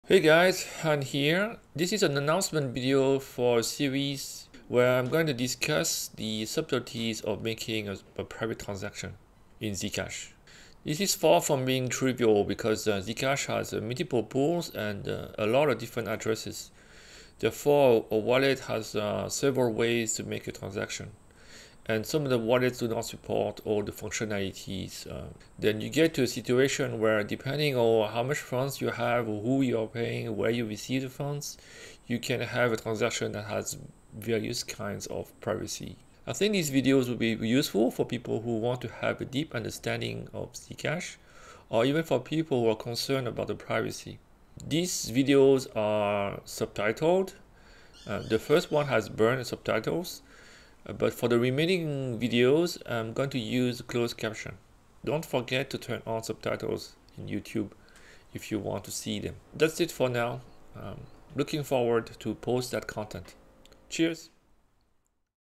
Announcement: Video Series on Transaction Privacy
Hey guys, Hanh here.

This is an announcement video for a series where I'm going to discuss the subtleties of making a private transaction in Zcash.

This is far from being trivial because Zcash has multiple pools and a lot of different addresses. Therefore, a wallet has several ways to make a transaction, and some of the wallets do not support all the functionalities. Then you get to a situation where depending on how much funds you have, who you are paying, and where you receive the funds, you can have a transaction that has various kinds of privacy.

I think these videos will be useful for people who want to have a deep understanding of Zcash, or even for people who are concerned about privacy.

These videos are subtitled. The first one has burned subtitles. But for the remaining videos, I'm going to use closed caption. Don't forget to turn on subtitles on YouTube if you want to see them. That's it for now. Looking forward to posting that content.

Cheers!